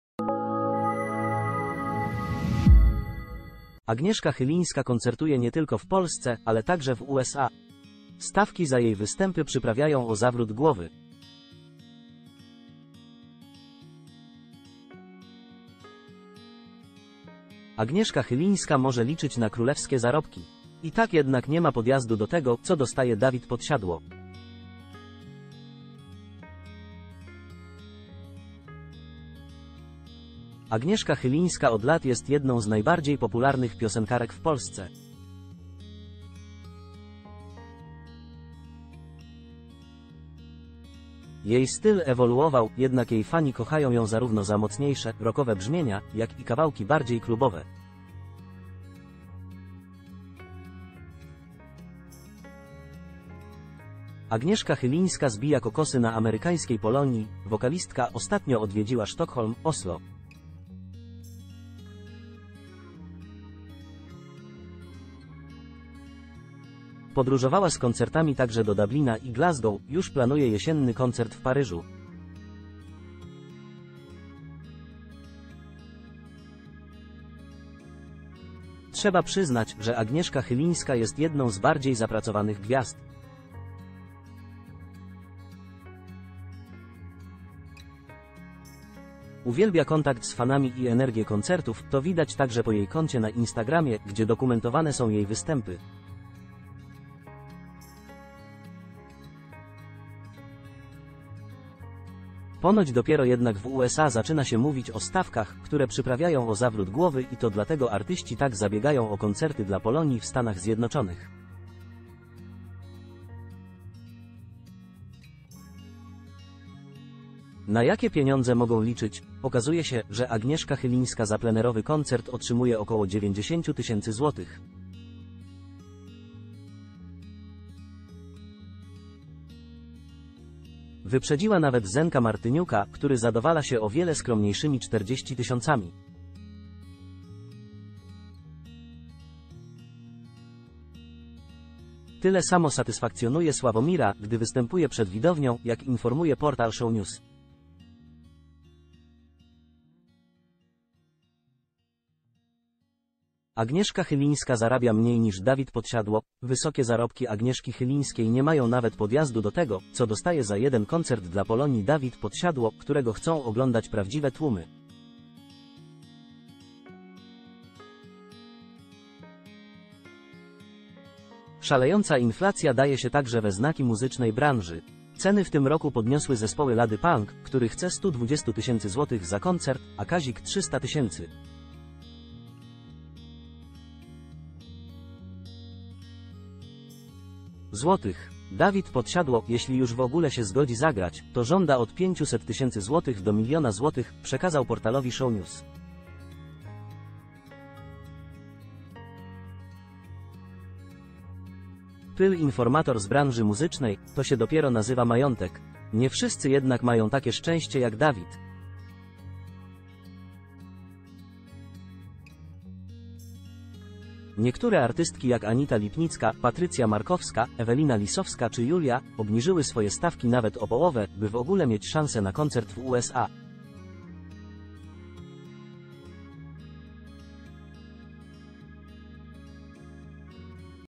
Agnieszka Chylińska dostaje fortunę za występ. Podsiadło i tak ją wyprzedził
Agnieszka Chylińska koncertuje nie tylko w Polsce, ale także w USA. Stawki za jej występy przyprawiają o zawrót głowy. Agnieszka Chylińska może liczyć na królewskie zarobki. I tak jednak nie ma podjazdu do tego, co dostaje Dawid Podsiadło.
Agnieszka Chylińska od lat jest jedną z najbardziej popularnych piosenkarek w Polsce. Jej styl ewoluował, jednak jej fani kochają ją zarówno za mocniejsze, rockowe brzmienia, jak i kawałki bardziej klubowe.
Agnieszka Chylińska zbija kokosy na amerykańskiej Pol  ...